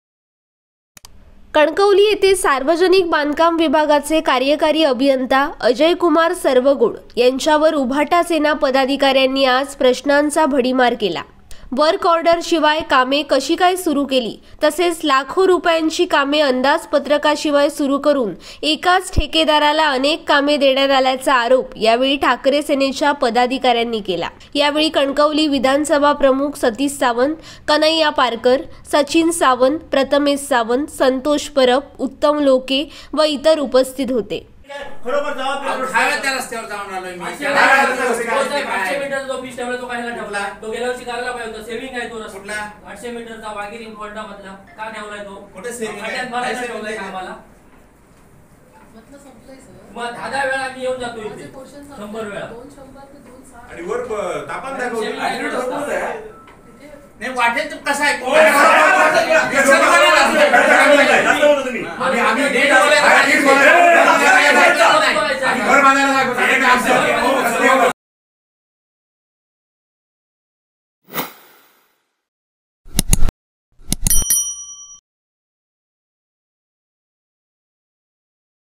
बांधकाम अभियंता अजयकुमार सर्वगोड यांना उबाठा सेनेने धरले धारेवर
Ads :